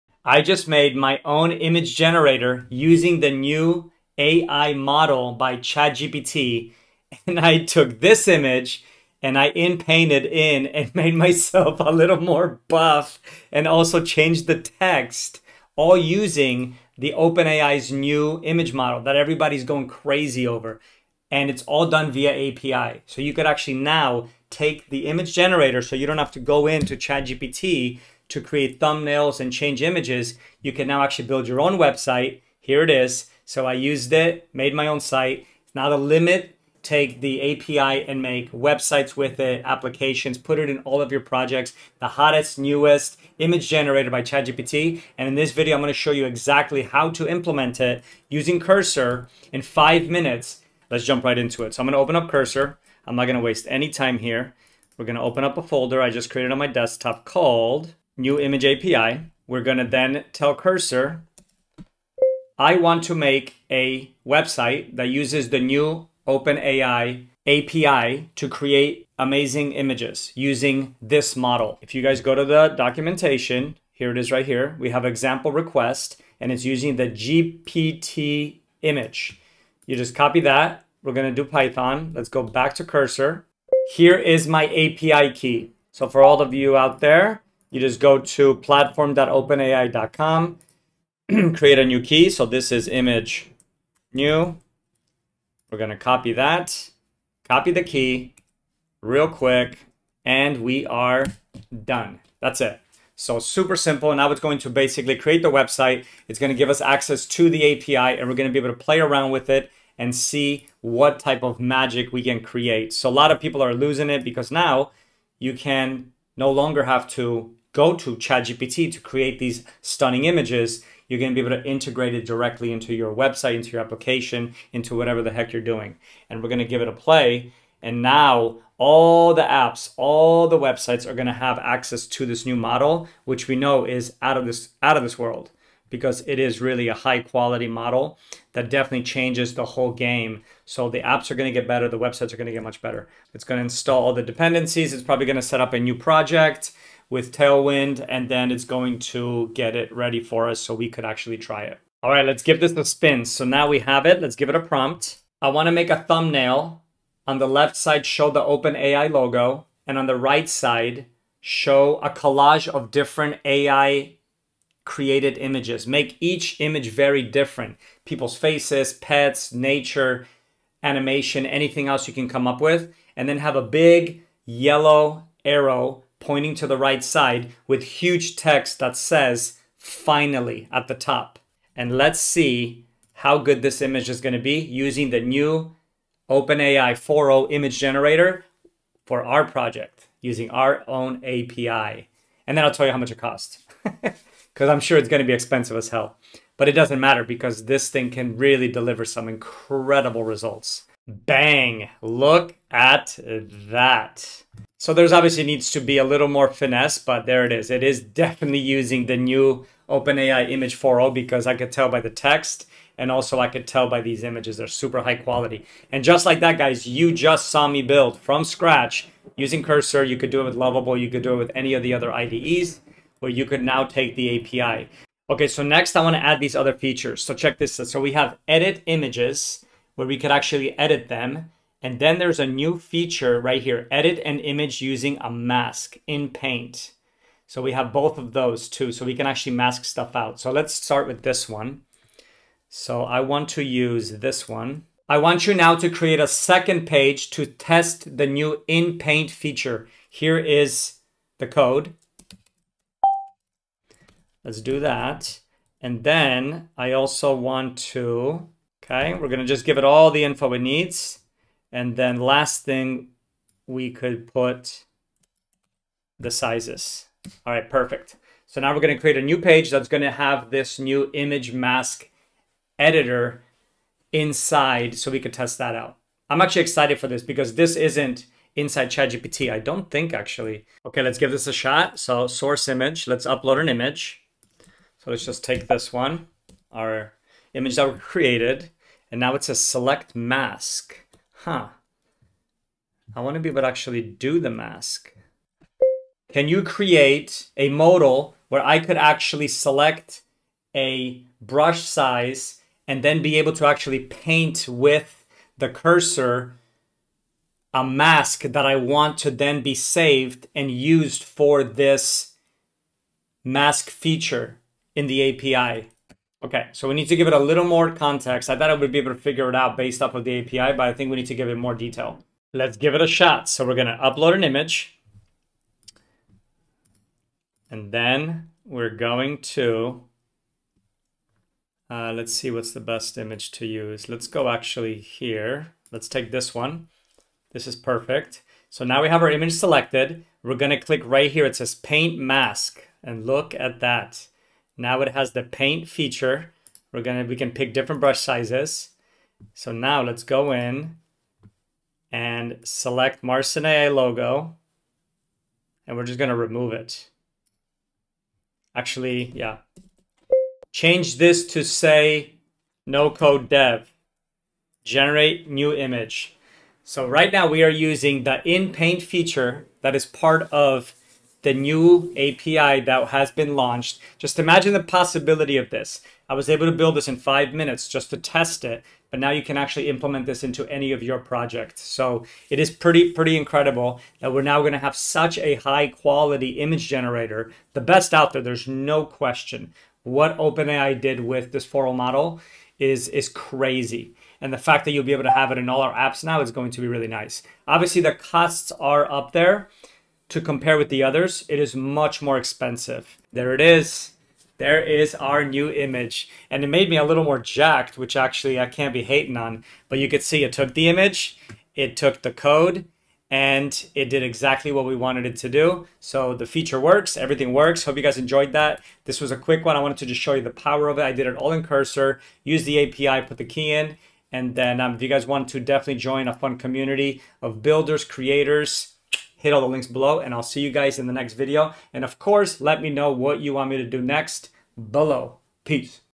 Apps will never be the same
The new ChatGPT 40 Image Generator API is live and it’s a total game changer for anyone building apps, websites, or tools with AI. OpenAI just made it insanely easy to create high-quality, ultra-realistic AI images right inside your project, with almost no coding required. Want custom graphics for your app? Viral art for your site? Automated content for your business? Now you can do it all in seconds, using the same AI tech behind ChatGPT 40. In this video, I’ll show you exactly how the new image API works, what makes it better than anything else out there, and why you need to try it right now before your competitors do. If you’re a developer, designer, entrepreneur, or just love experimenting with AI, this is the launch you can’t miss. Watch the demo, see the results, and get inspired to build something wild with ChatGPT 40’s new image power.

Got question or want to collab?